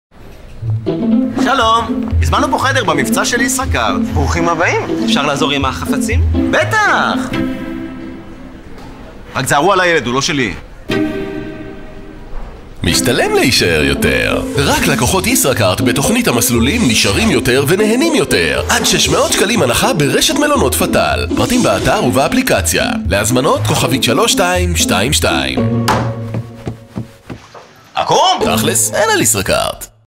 עד 600 ₪ ללקוחות ישראכרט ברשת מלונות פתאל
רק לקוחות ישראכרט מקבלים עד 600 ₪ הנחה ברשת מלונות פתאל! מזמינים עד ה-31.12.14 חופשה מפנקת באחד ממלונות פתאל לחודשים דצמבר-ינואר ונהנים מהנחות: 100 ₪ הנחה למזמינים לילה אחד, 300 ₪ הנחה למזמינים 2 לילות או 600 ₪ הנחה למזמינים 3 לילות.
*התשלום בהגעה למלון באמצעות כרטיס ישראכרט בלבד בן 8 או 9 ספרות או כרטיסים נוספים מקבוצת ישראכרט המתחילים בספרות הבאות: מסטרקארד – 5326; ויזה – 4557